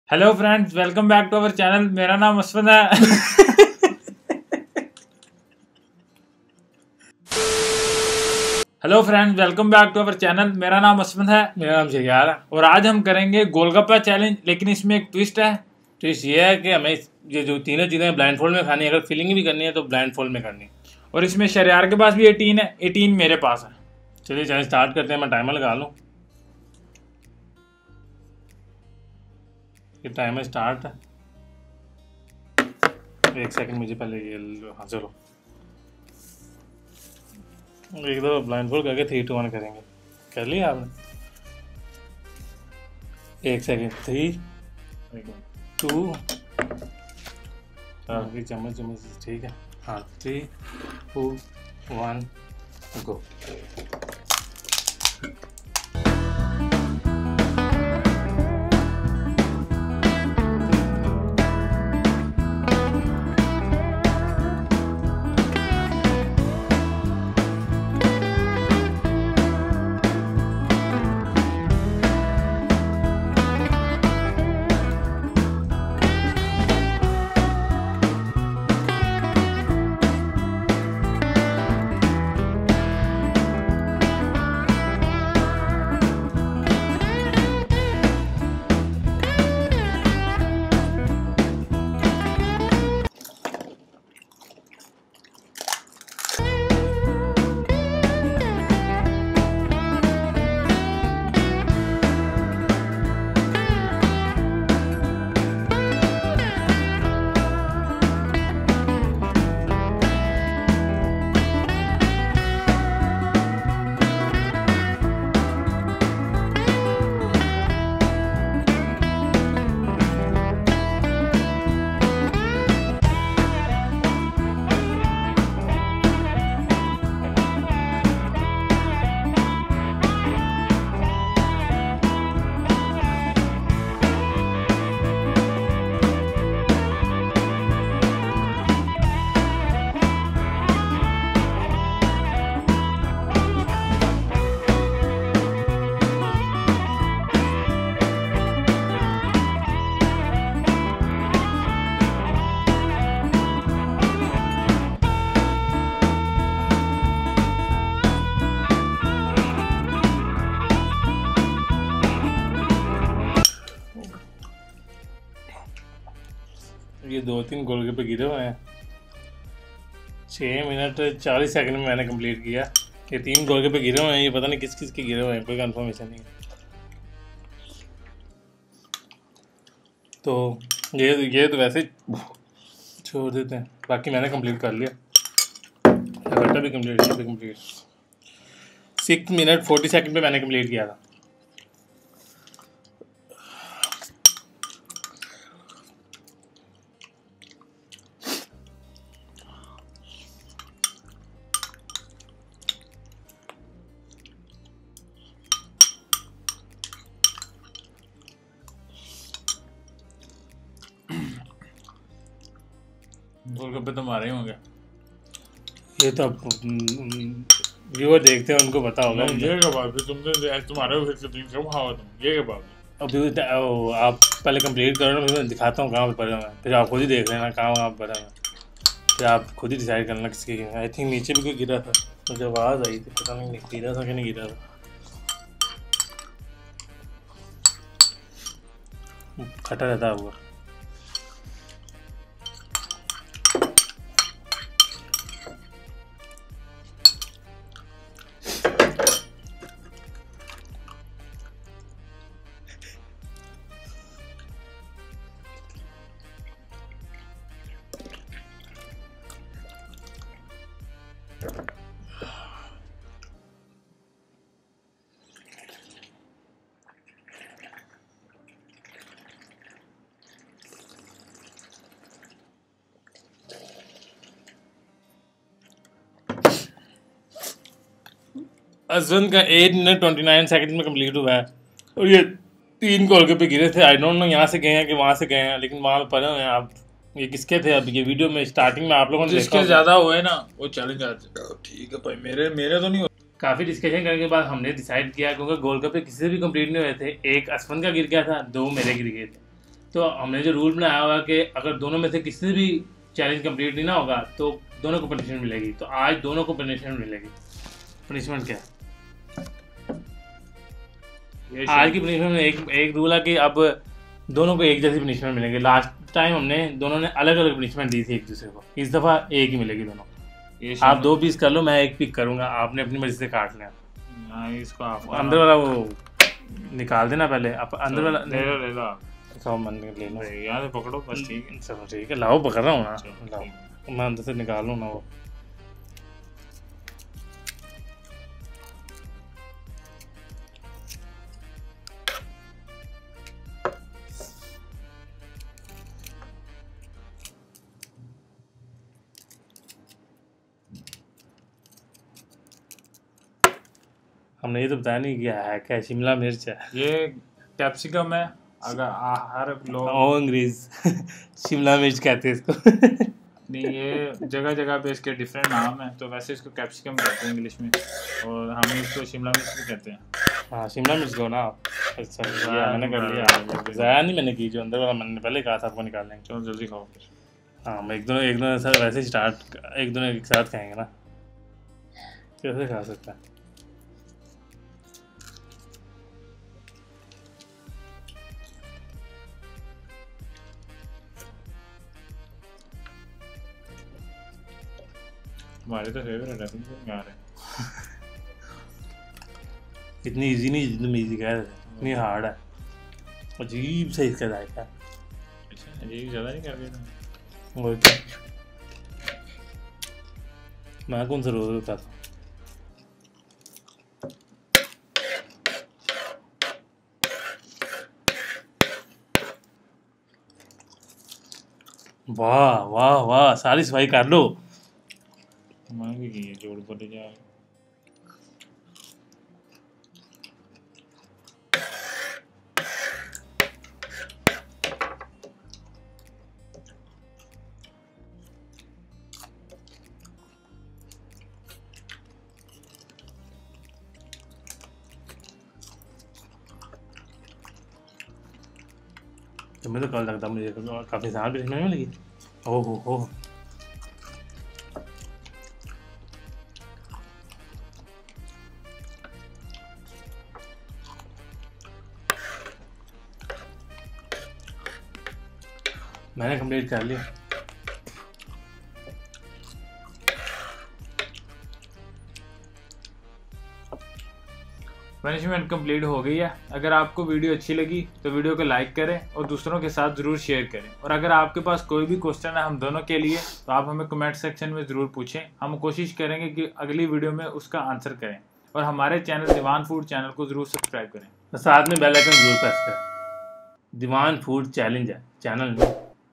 BLINDFOLDED GOLGAPPA / PANI PURI EATING CHALLENGE | Pani Puri Eating Competition | Food Challlenge
In this video me and my brother are going to eat pani puri / golgappa in blindfolded.